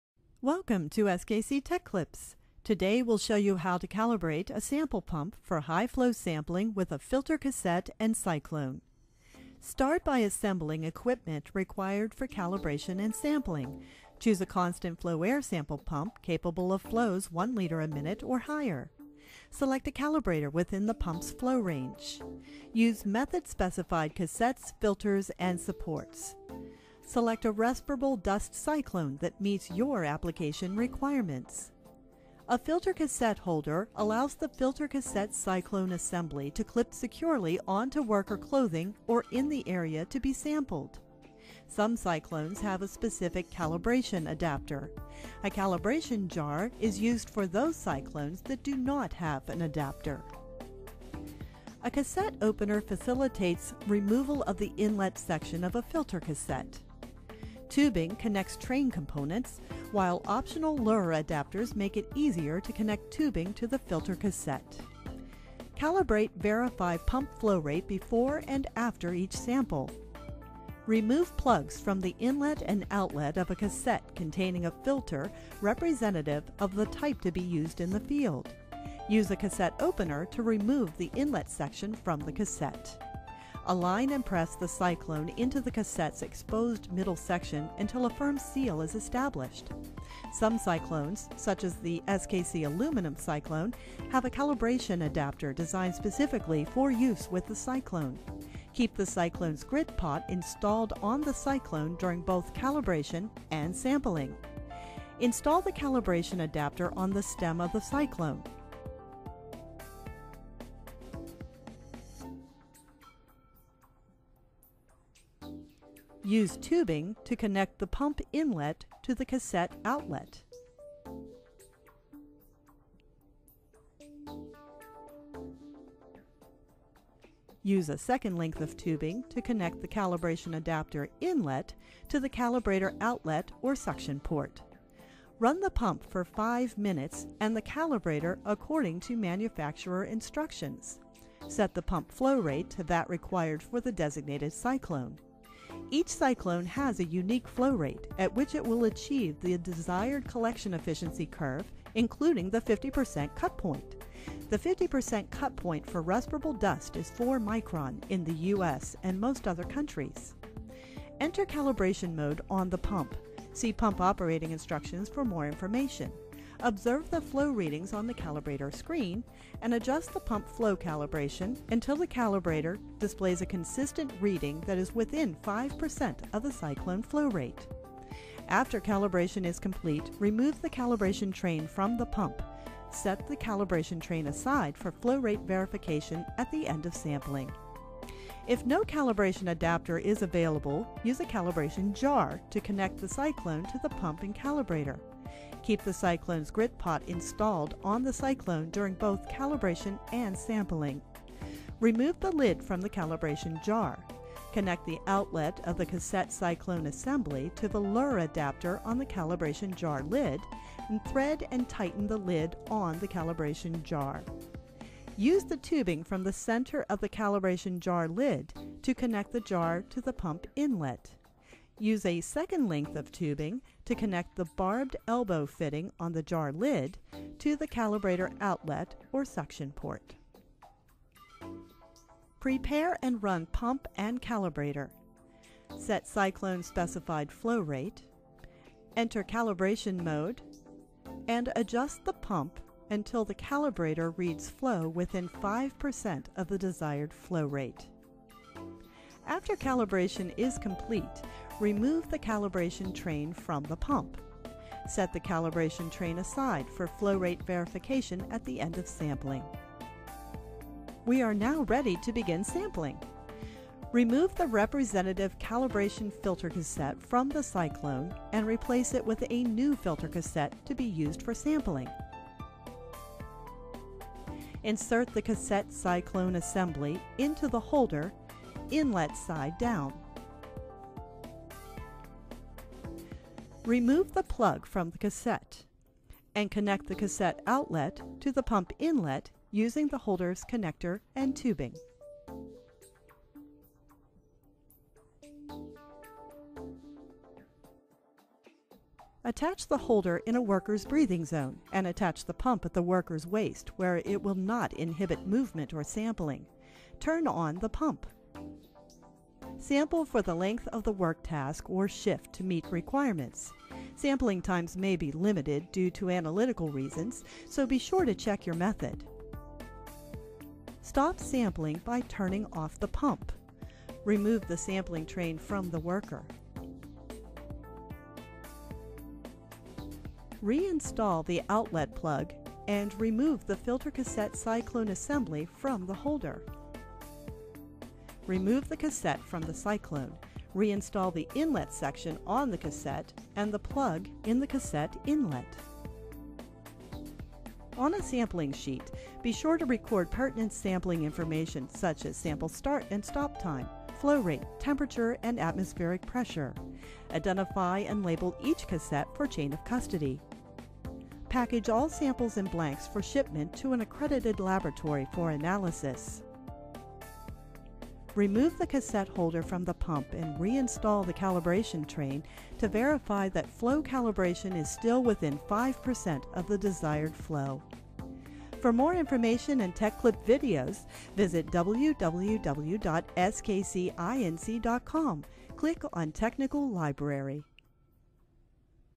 skc tech klip high flow calibration and sampling with filter cassette and cyclone
Filter cassetter and Cyclone for high flow sampling from SKC